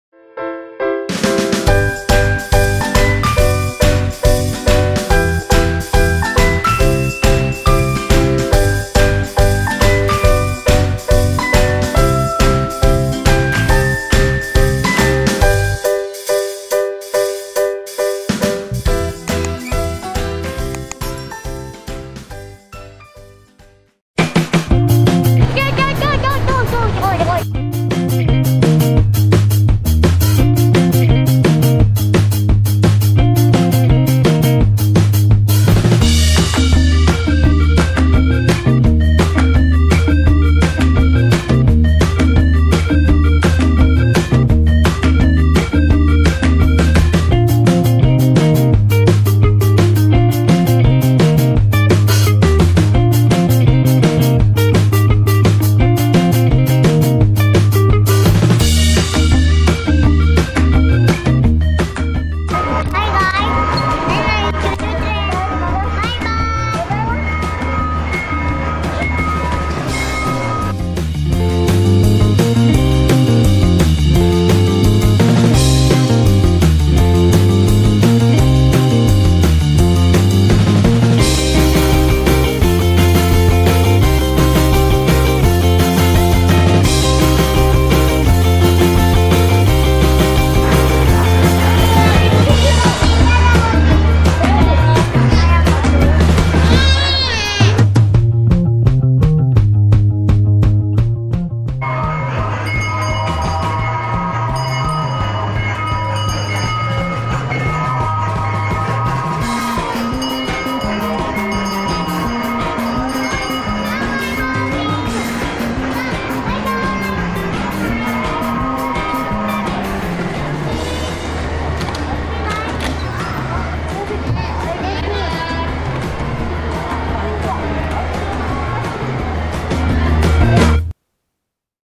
NAIK KERETA API - CHUCHU TRAIN
naik kereta api
lokasi di sun plaza medan

chuchu train indoor